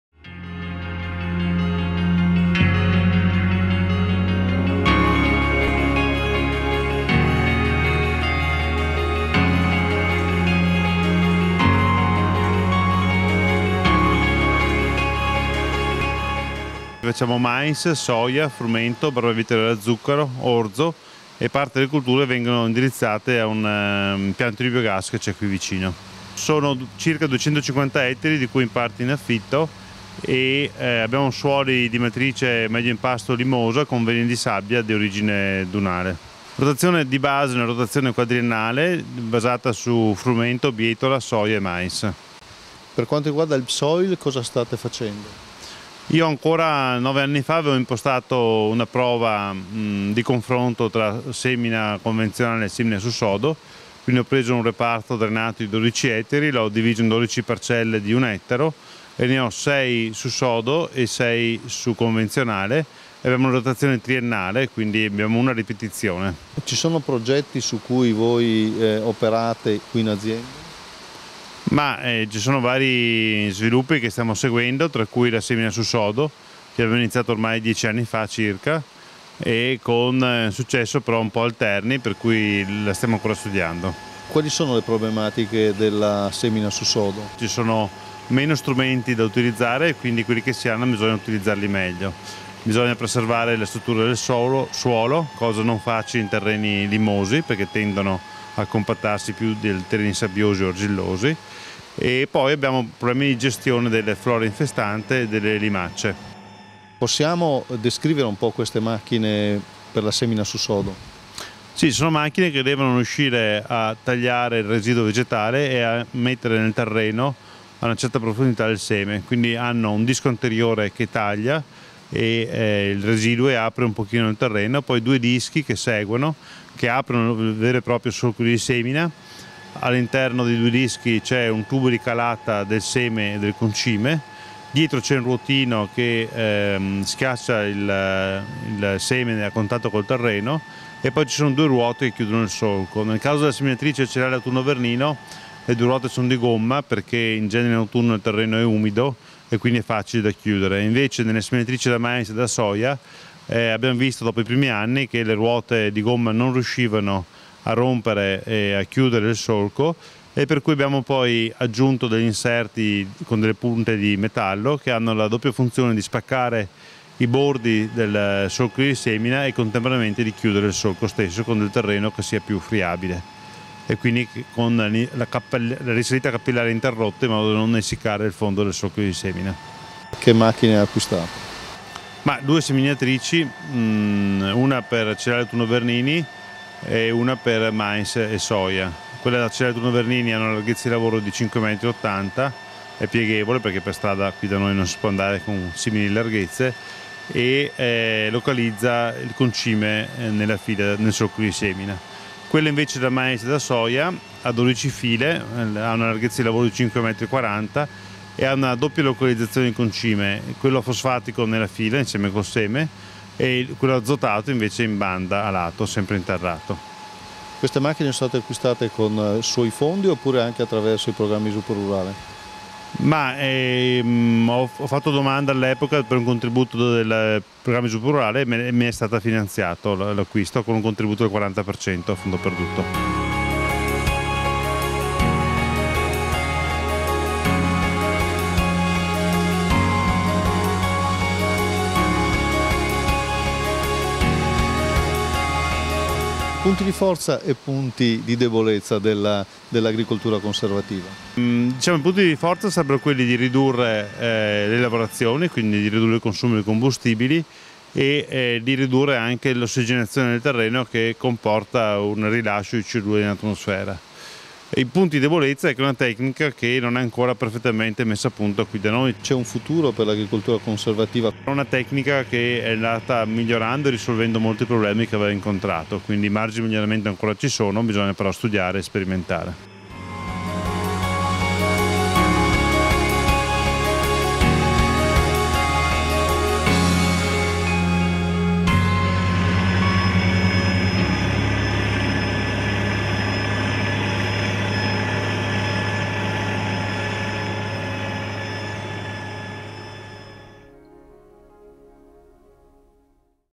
Progetto HelpSoil Azienda Agricola Livenzuola
Tecniche di semina su sodo presso l'azienda Marco Aurelio Pasti ad Eraclea (VE)